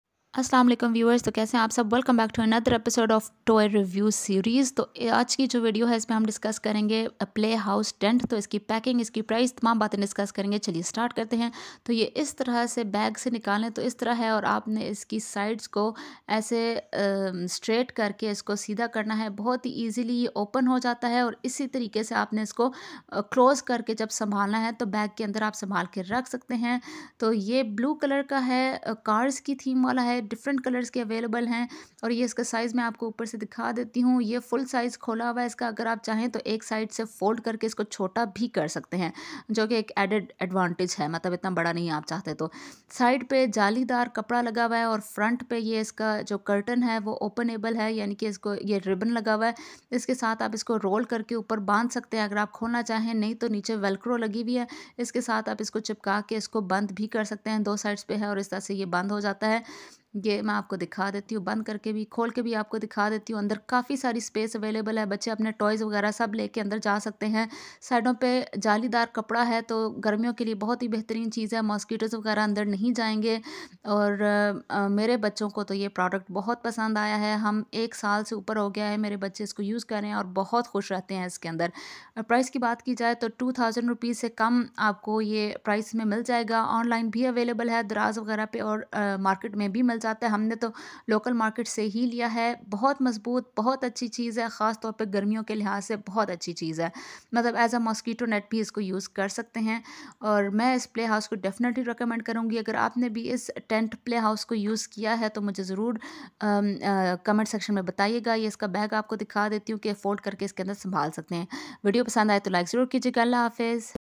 kids toy review series: tent playhouse, price, quality, worth buying?
we purchased this playhouse for our kids some time ago and now I want to share my review with you. hope you like the video